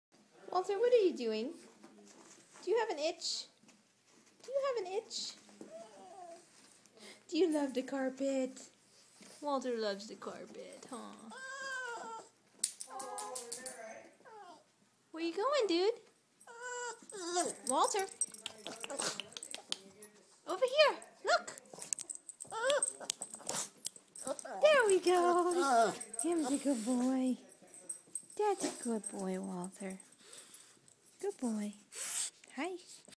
Walter has an itch
Walter uses the carpet to scratch